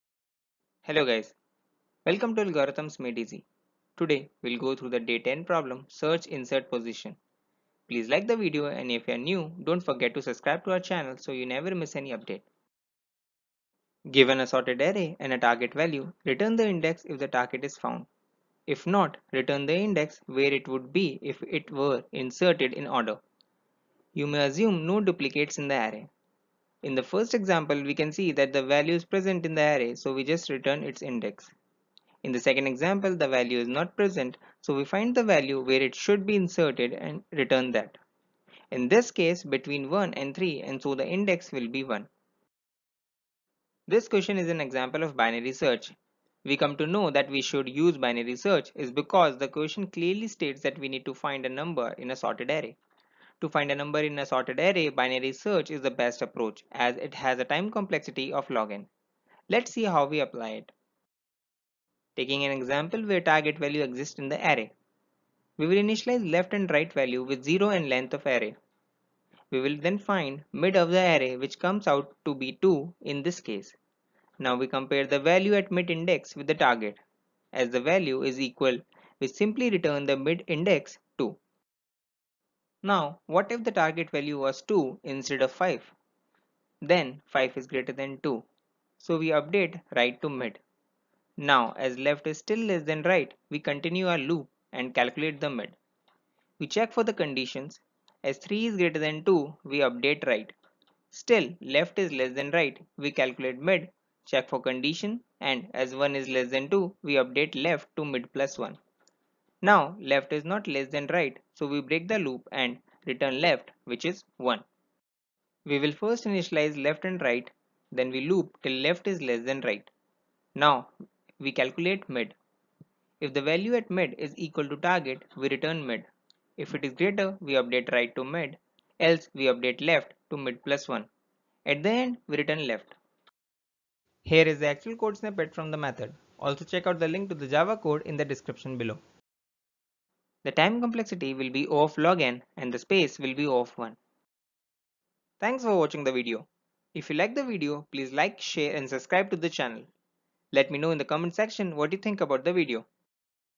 Search Insert Position | Day 10 | [June LeetCoding Challenge] [Leetcode #35] [2020]
The day 10 problem in June Leetcoding Challenge. ( Search Insert Position ).

Problem statement:
Given a sorted array and a target value, return the index if the target is found. If not, return the index where it would be if it were inserted in order.

You may assume no duplicates in the array.

Example 1:
Input: [1,3,5,6], 5
Output: 2

Example 2:
Input: [1,3,5,6], 2
Output: 1

Example 3:
Input: [1,3,5,6], 7
Output: 4

Example 4:
Input: [1,3,5,6], 0
Output: 0